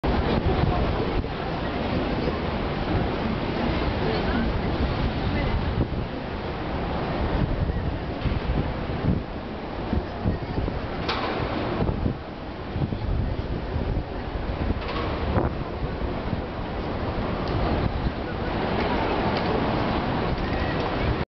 OSAKA STATION CITY
WALK DOWN FROM PASSAGE(3RD  FLOOR) TO ATRIUM HIROBA,OSAKA STATION CITY